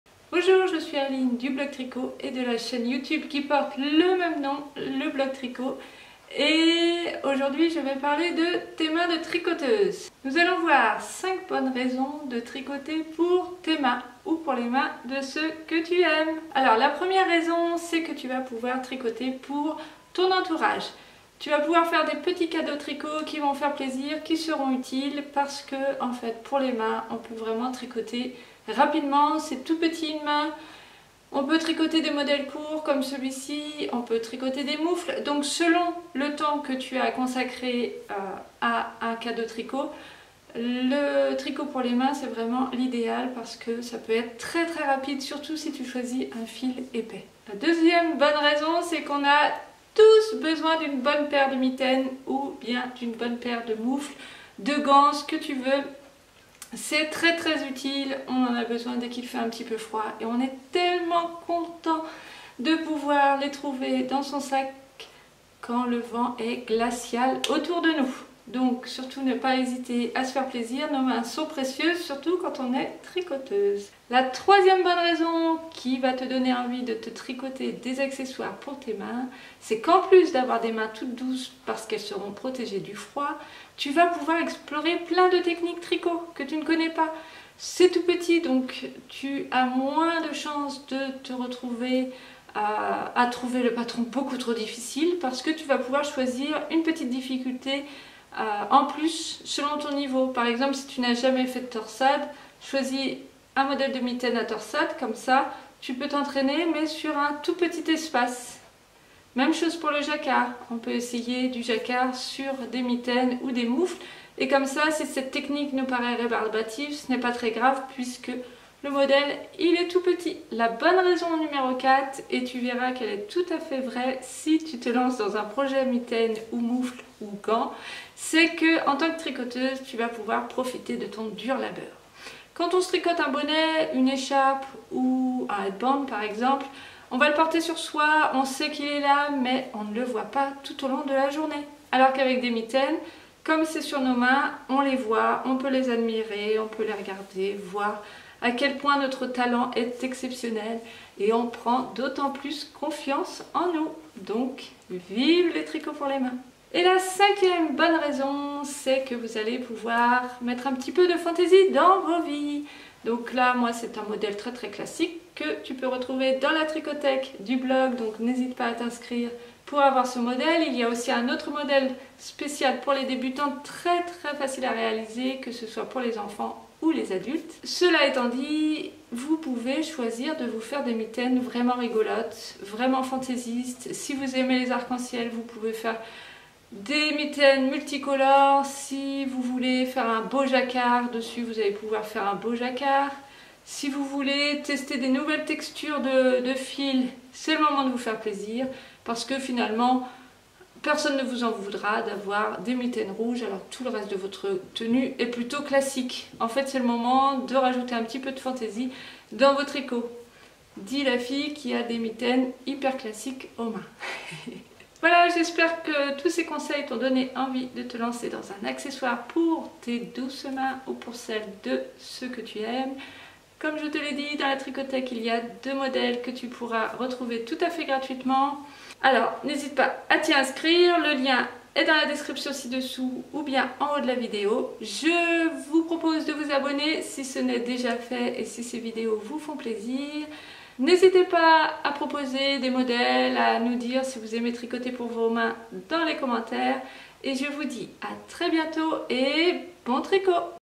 5 bonnes raisons de tricoter pour nos mains :)
Tu peux retrouver deux modèles de mitaines sur le blog et dans la tricothèque.
De quoi faire de jolis présents tricotés main pour les mains de ceux que vous aimez :)